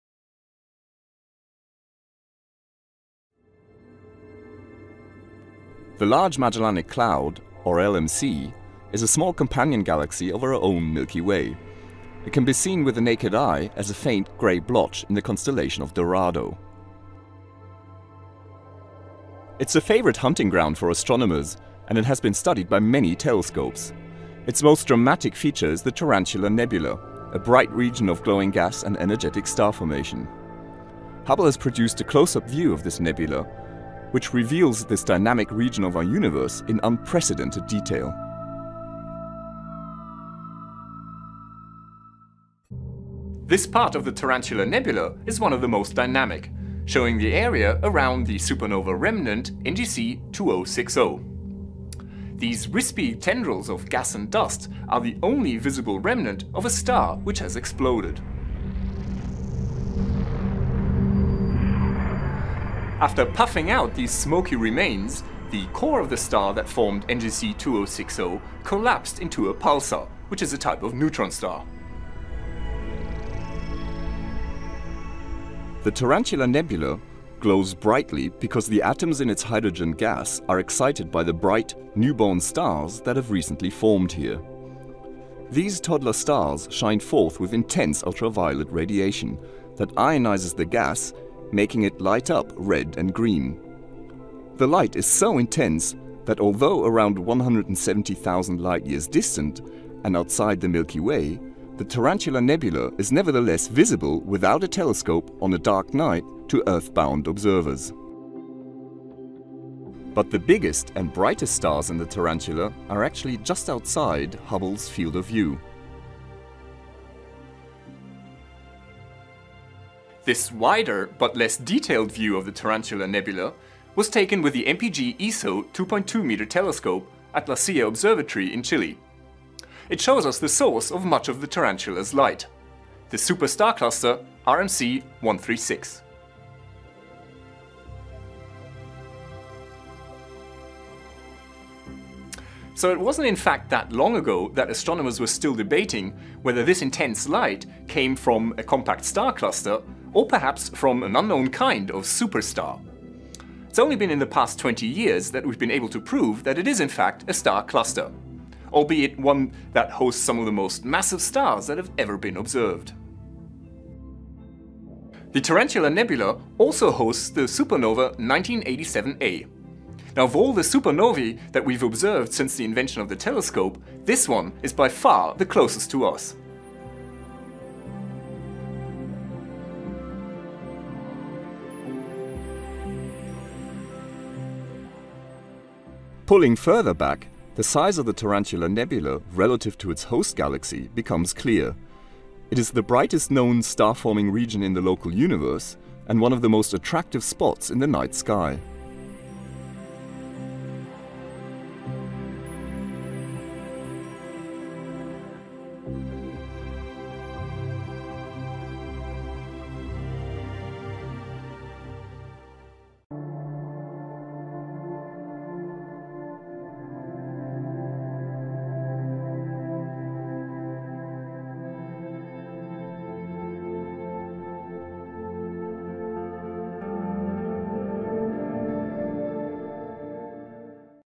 Tour of the Tarantula Nebula
Joe Liske (Dr J) takes us on a tour of the Tarantula Nebula. Bright star forming gas clouds, super star clusters and supernova remnants are just some of the sights in this dramatic region of the night sky.